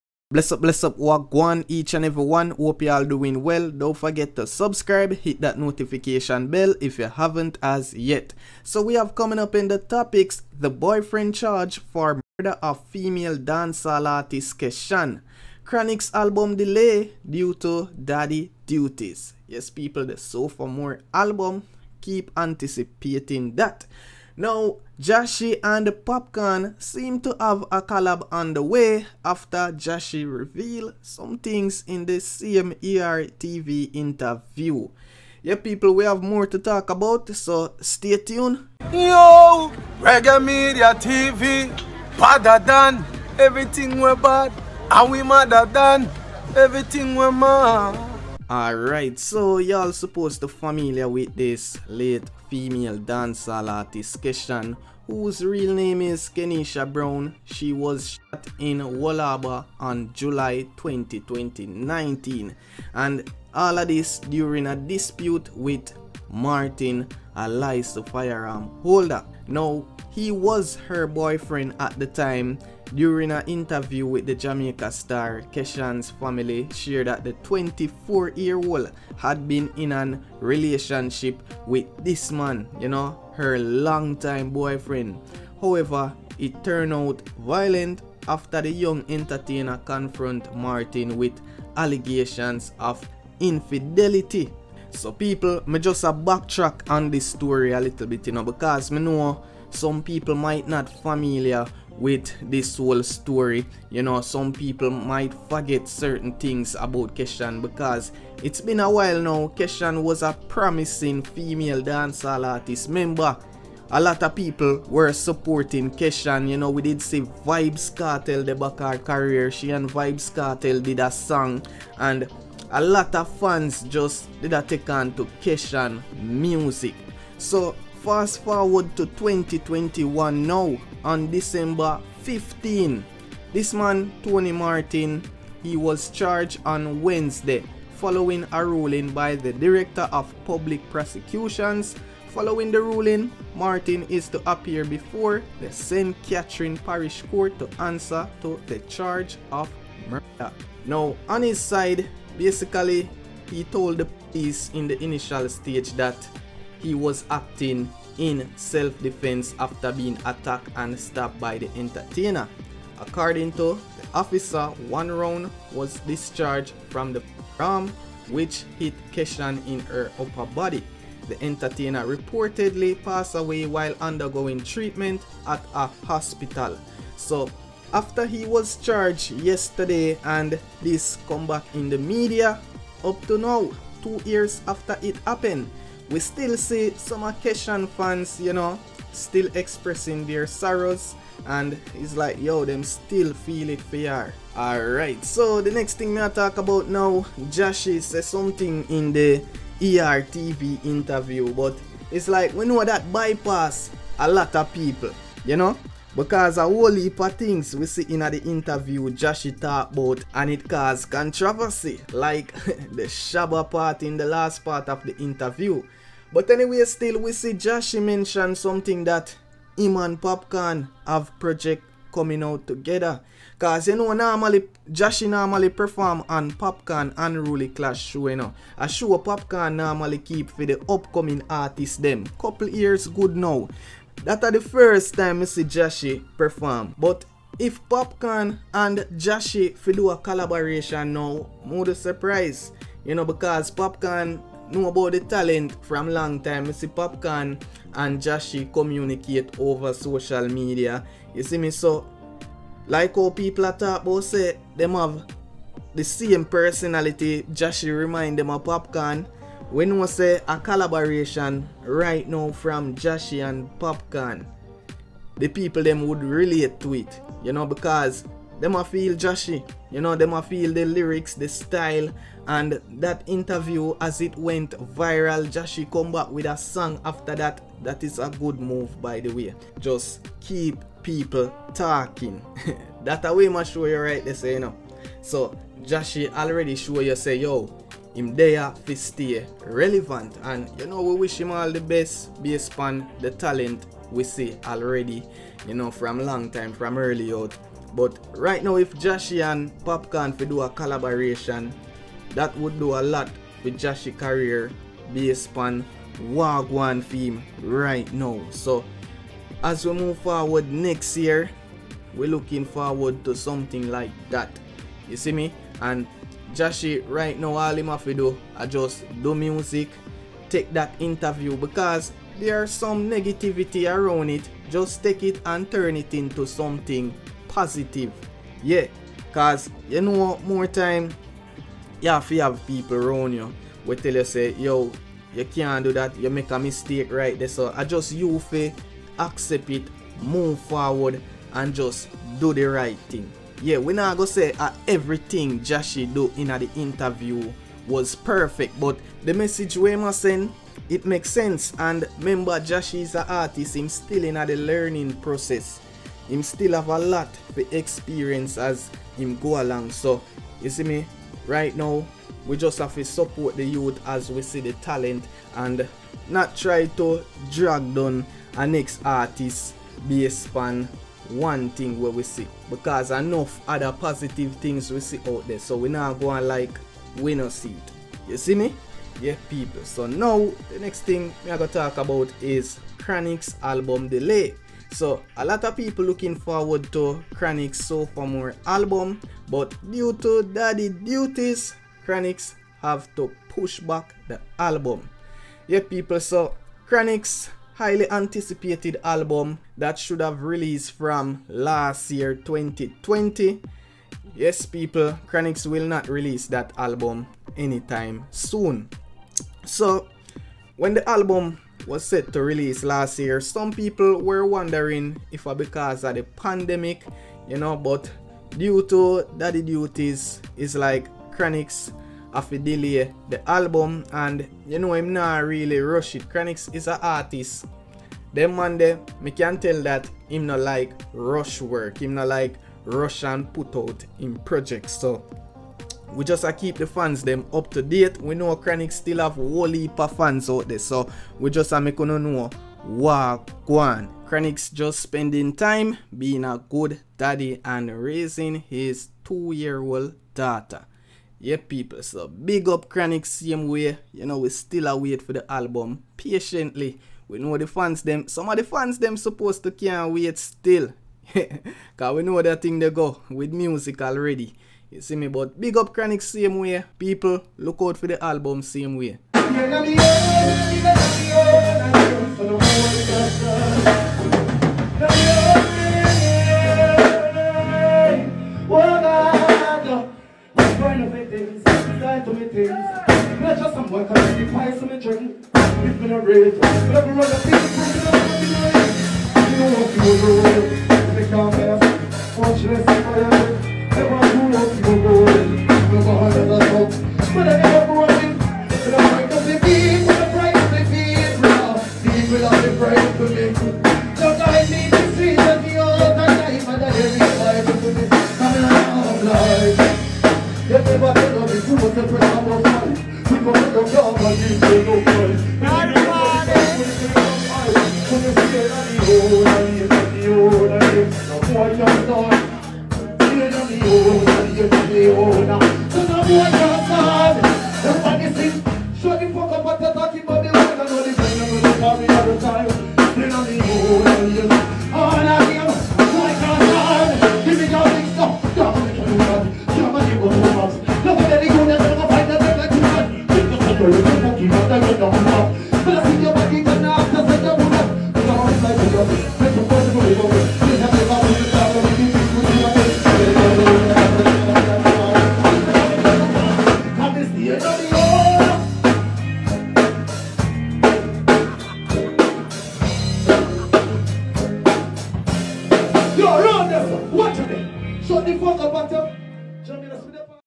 Breaking! Dancehall Artist K!||ed, Her Bf Charge For MVRD£R | Jahshii & Popcaan Callab | Chronixx
Keshan, whose real name is Kenneisha Brown, was shot in Old Harbour on July 20, 2019, during a dispute with Martin, a licensed fire@rm holder.
According to the p0|ice, one round was discharged from the fire@rm which hit Keshan in her upper body.
The entertainer reportedly d!ed while undergoing treatment at a hospital.